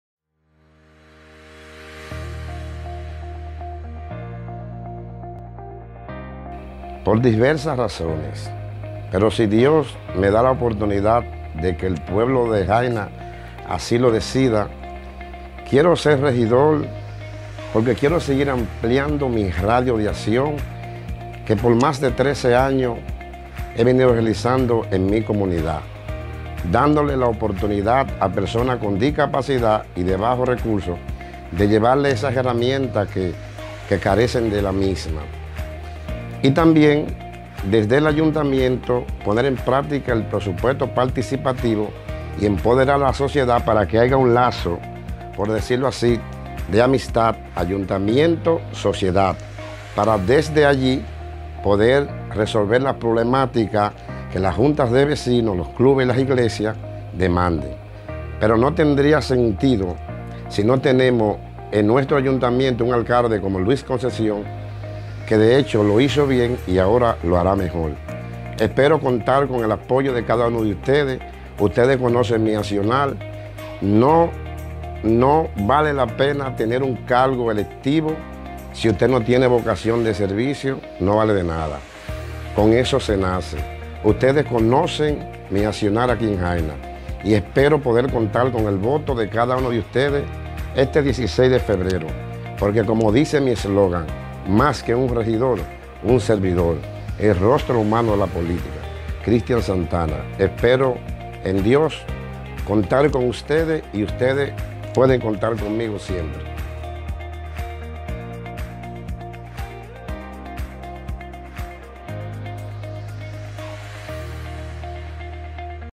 PORQUE VOTAR POR MI – CRISTIAN SANTANA
En nuestra sección dedicada a los candidatos a posiciones políticas para esta contienda del 16 de febrero del presente año. Para participar escríbenos por whatsapp al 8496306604